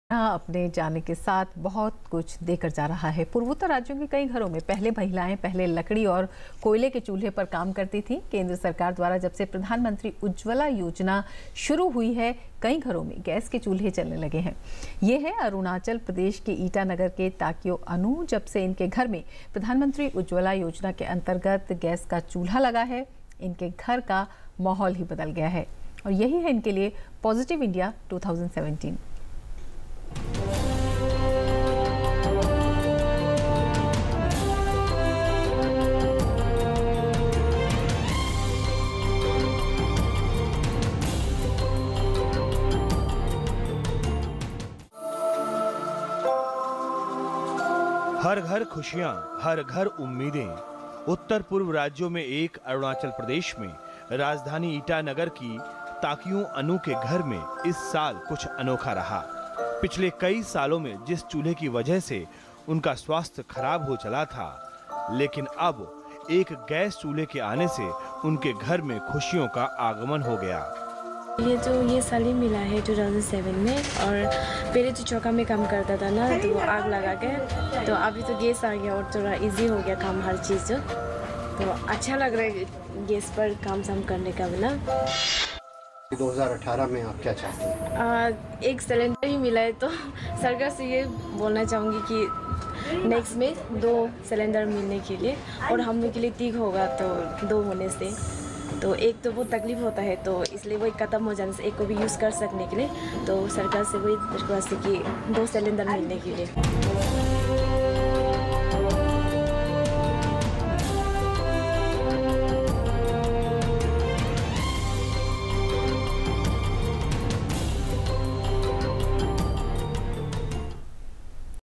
Positive India : पूर्वोत्तर राज्यों में घर में गैस चूल्हा आने से बदल गया जीवन
साल 2017 अपने जाने के साथ बहुत कुछ दे कर जा रहा है । पूर्वोत्तर राज्यों के कई घरों में पहले महिलाएं पहले लकड़ी और कोयले के चूल्हे पर काम करती थी।  केंद्र सरकार द्वारा जब से 'प्रधानमंत्री उज्जवला योजना' शुरु हुआ है, कई घरों में गैस के चुल्हें जलने लगें हैं। ये हैं अरुणाचल प्रदेश के ईंटानगर के ताकियों अनू। जब से इनके घर में प्रधानमंत्री उज्जवला योजना के अन्तर्गत गैस का चुल्हा लगा है इनके घर का माहौल ही बदल गया है। यही है उनके लिए POSITIVE INDIA 2017...